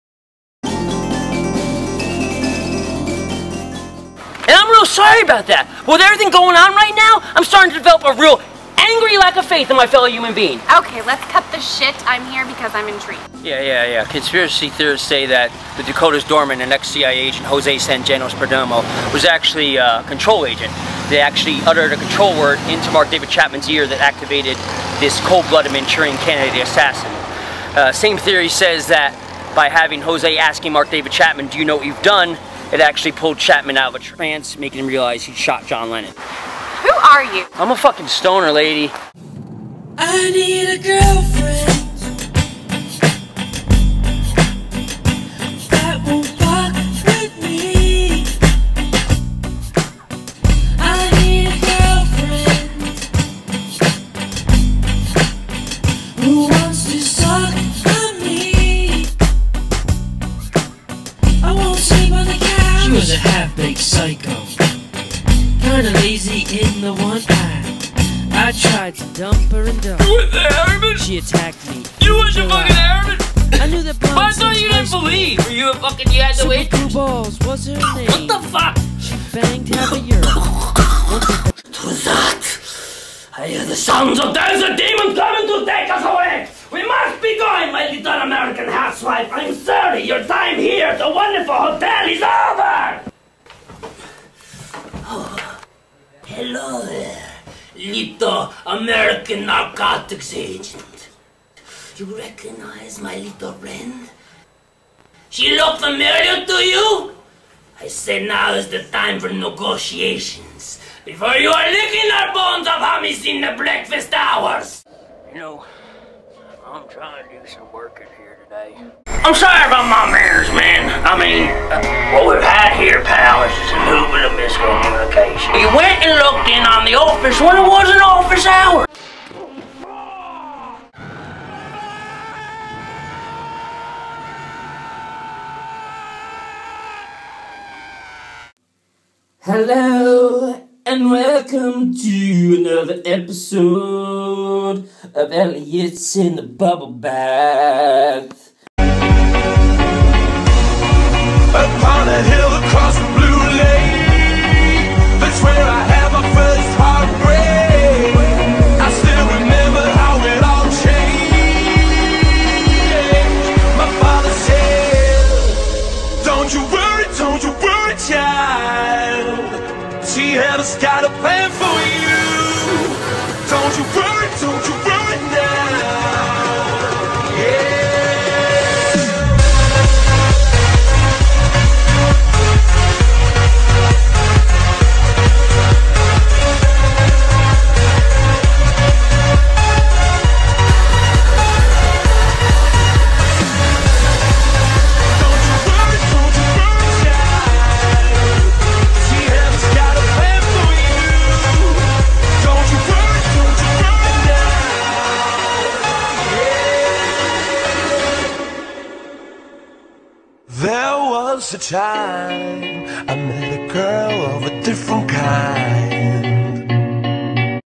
THE ELLIOT PASSANTINO ACTING RAPPING REEL
A quick snippet of the best of Elliot Passantino in film, Tv and as rapper Uncle Dirty. Too long for professional agencies? I say nay! How else can you cram over 7 years of ingenius character acting, improv dance and rapping into a reel???